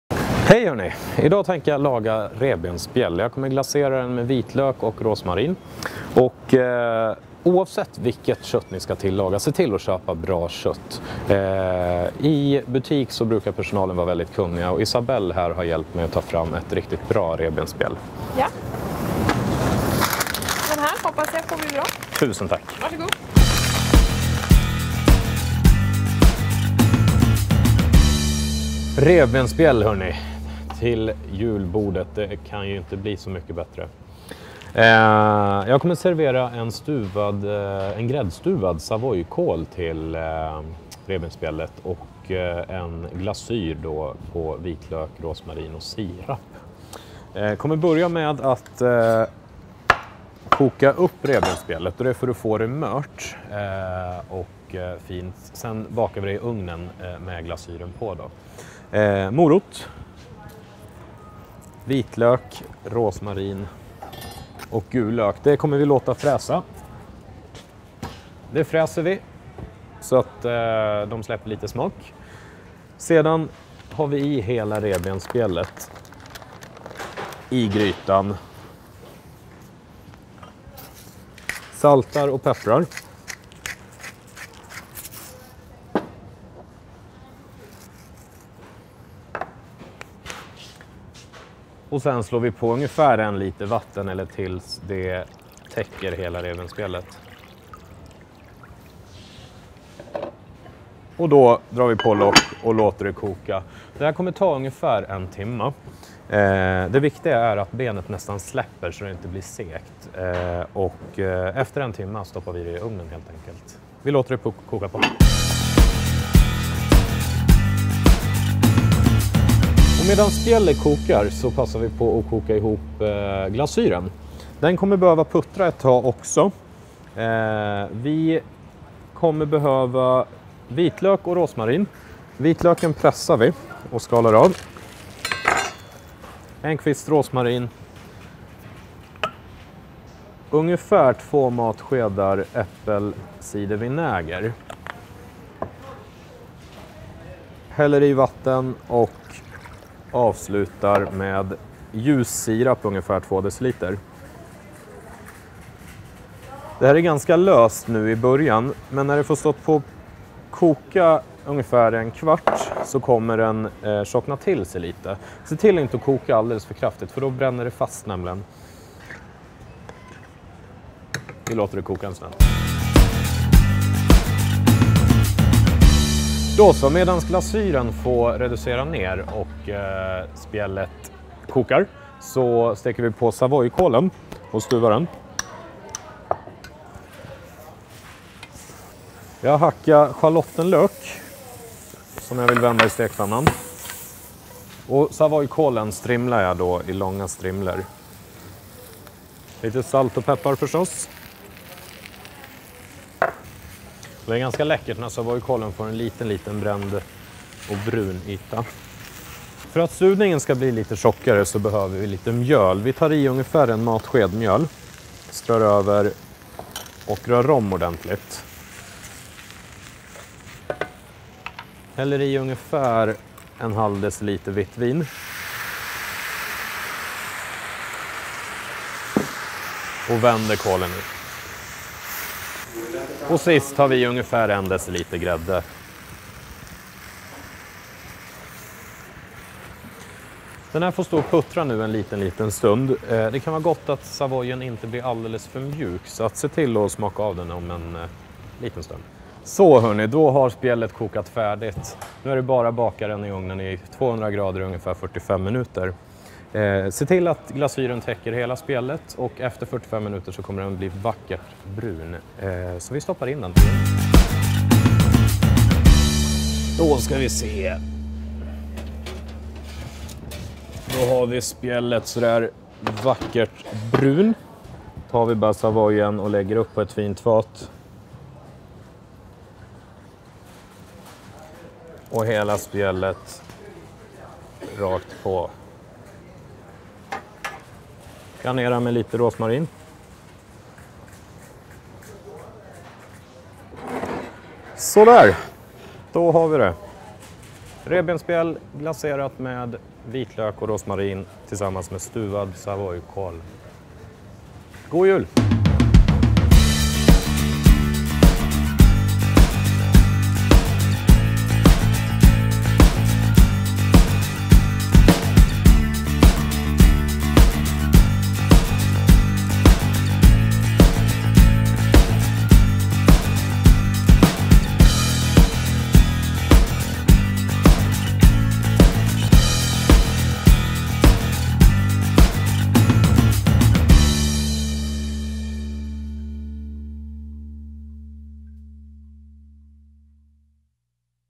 Löjligt goda ribs till julbordet
ANNONS. Mat-tv med Chabbe. Så gör du perfekta tillbehöret.
Chabbe Gabbour ger dig perfekta mattips inför julen.
Det här programmet är den tredje delen av fyra i en serie med mattips, perfekta för jul och nyår.